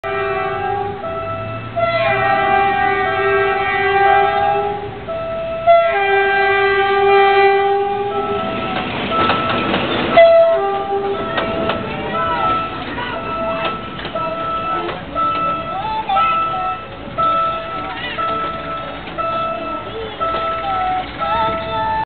DEL 922 Hauling EMU 203 set 2..
Passing San Andres Railcrossing Manila